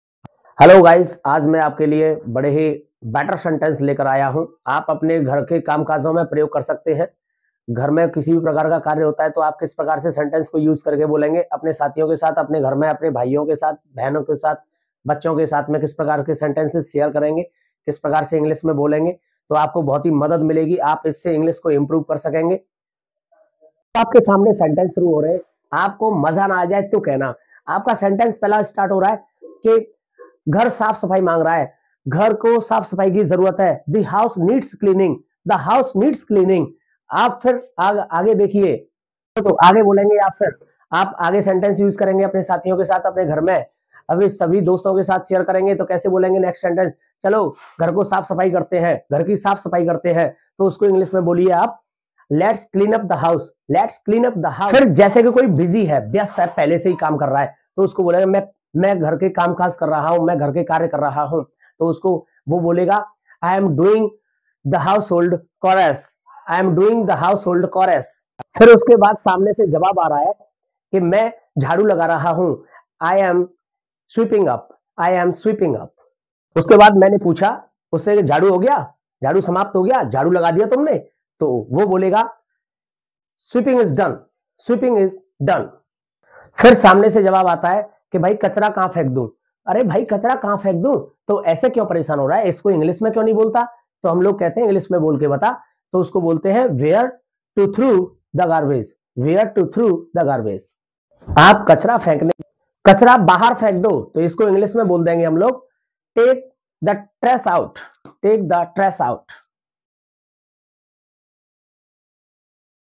daily uses sentences|| रोजाना बोले जाने वाले
dailyenglish Ghar Mein rojana Bole jaane wale Vakya,ghar ki saf safari me use hone wale sentence